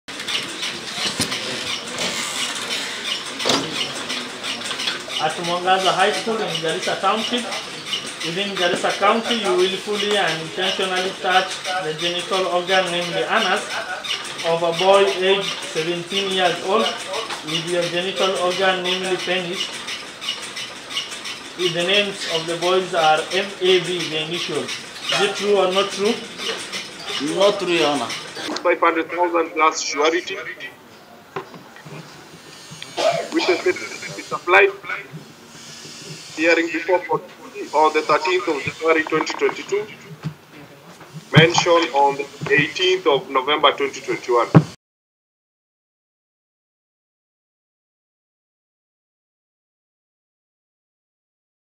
Mwangaza School executive director arraigned in court over sodomy claim
The Executive Director of Mwangaza Schools, Hussein Hassan Ali, in Garissa town, was today arraigned before a Garissa court charged with sodomizing a seventeen-year-old boy.